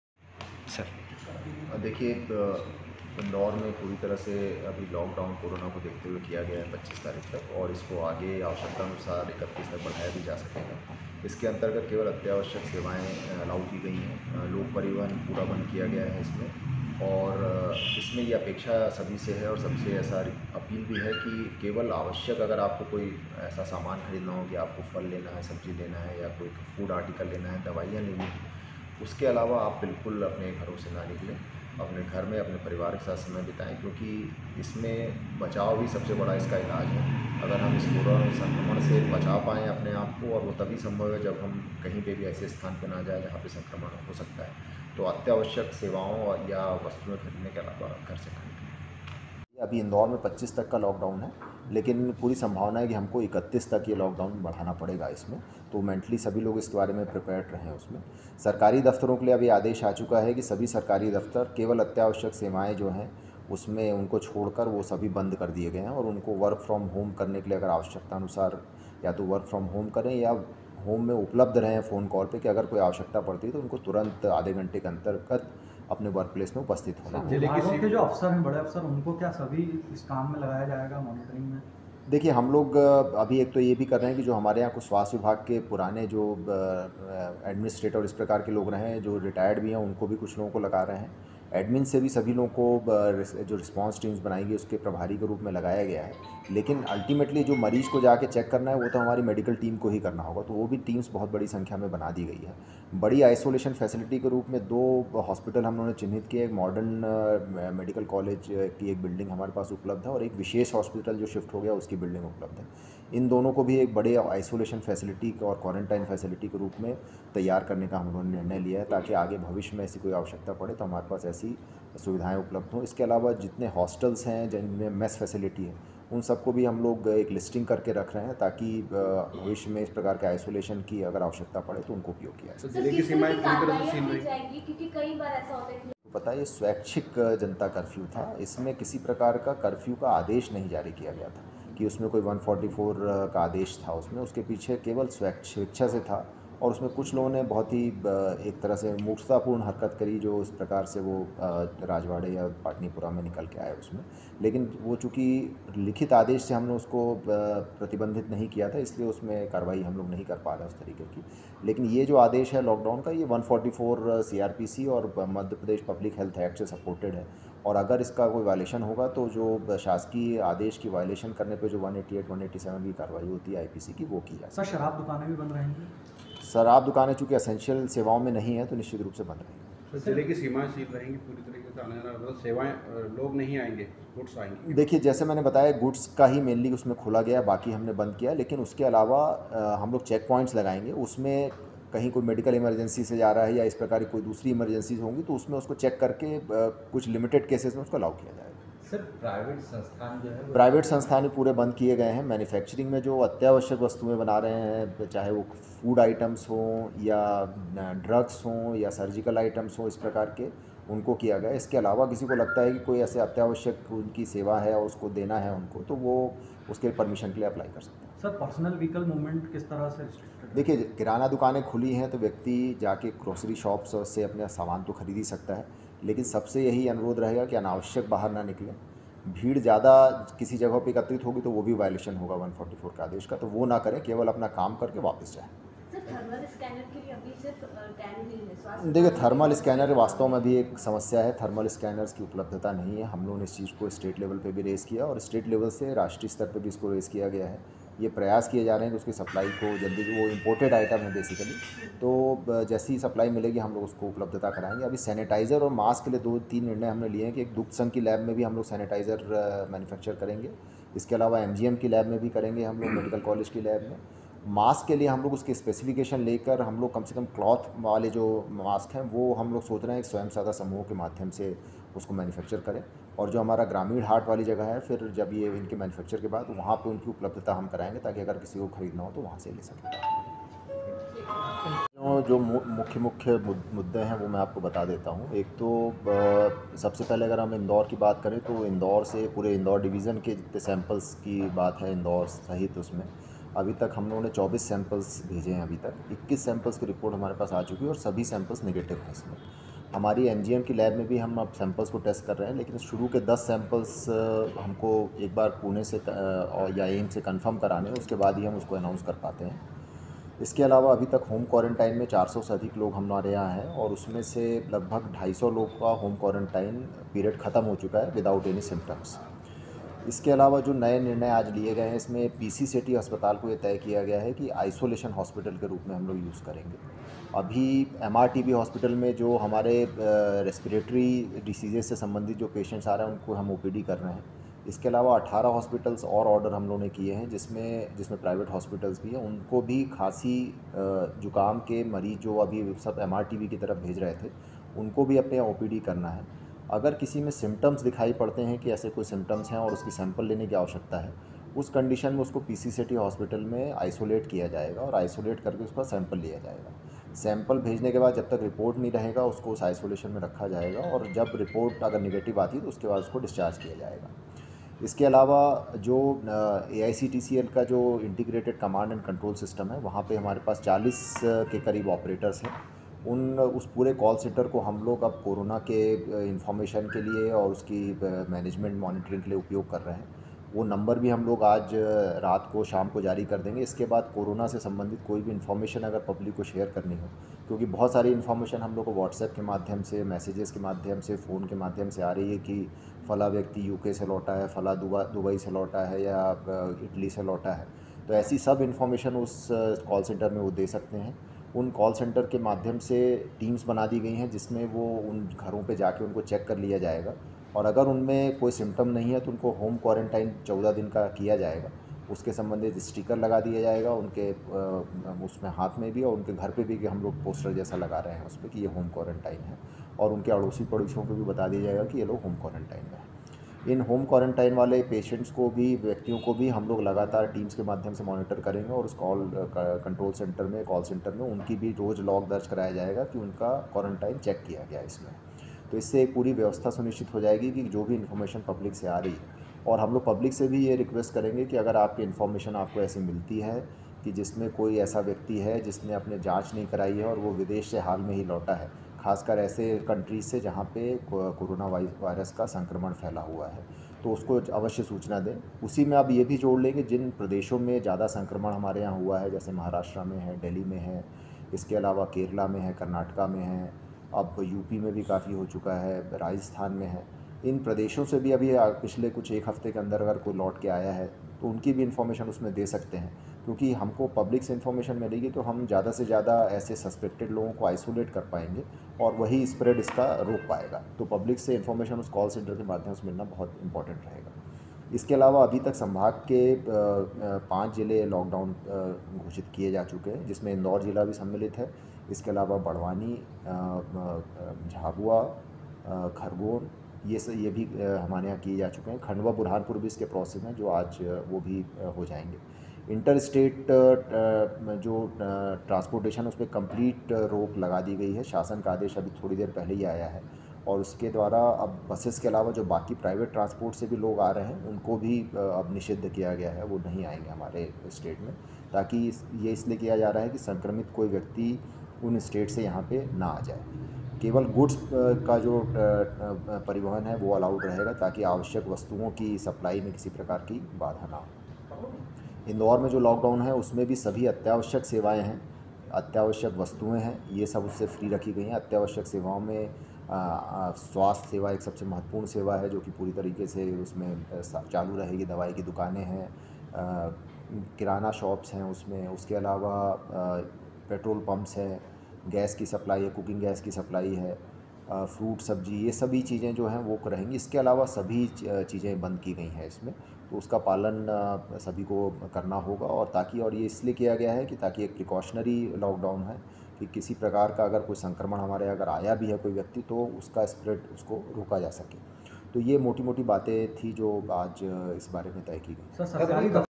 25 मार्च तक इंदौर हुआ लॉक डाउन बढ़ सकती है समय सीमा
25 मार्च तक इंदौर हुआ लॉक डाउन बढ़ सकती है समय सीमा